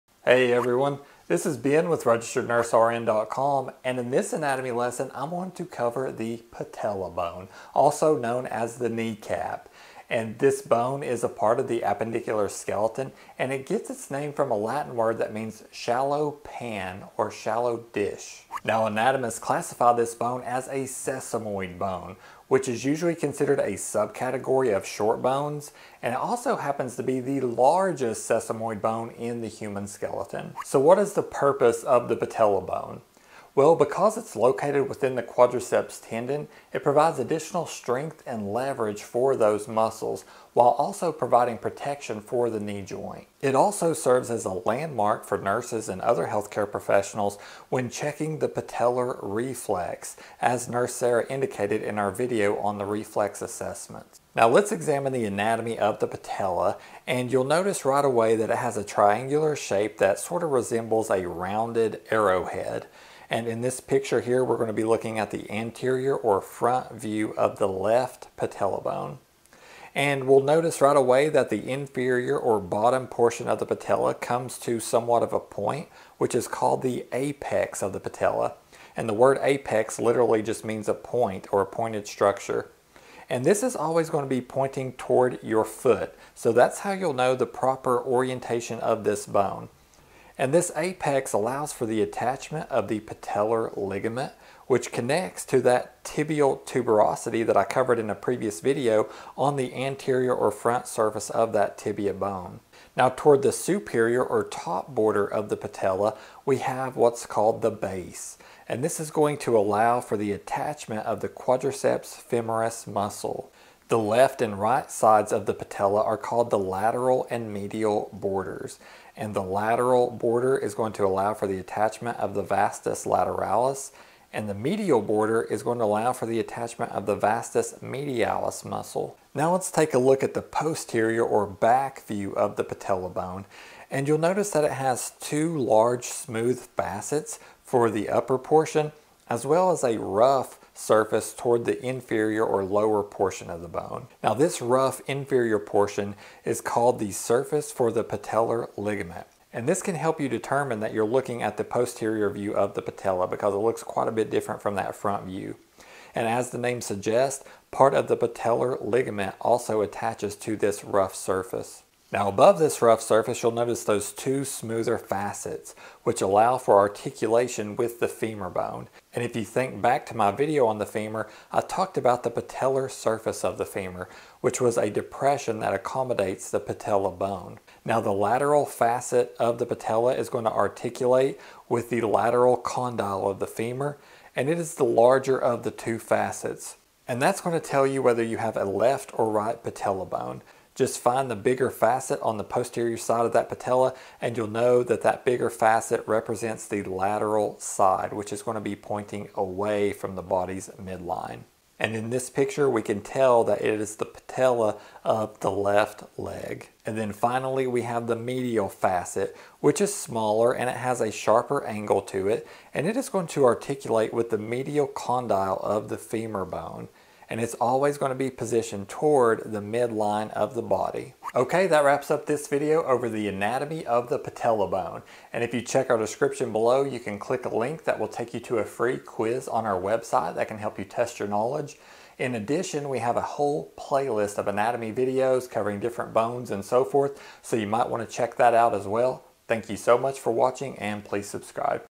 Patella Anatomy | Patella Kneecap Bone Review
Patella anatomy video with diagram - Hello, everyone. In this anatomy lesson, I'm going to cover the anatomy of the patella bone, better known as the kneecap.

This bone is part of the appendicular skeleton, and anatomists classify this bone as a sesamoid bone.

In this video, I'll be looking at the main characteristics of the patella bone, and I'll examine both the anterior (front) view, as well as the posterior (back) view.

The key structures you'll need to know for most undergraduate anatomy courses include the following:

Anterior side:

-Base
-Apex
-Medial border
-Lateral border

Posterior side:

-Surface for patellar ligament
-Lateral facet for lateral condyle of the femur
-Medial facet for medial condyle of the femur